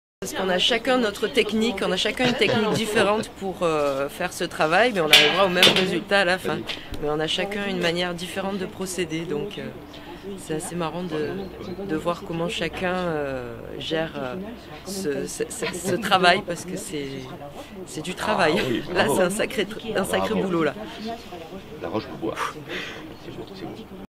10 h 10 : Stephanie explique le travail - Académie Balzac
"On a chacun une manière différente de procéder"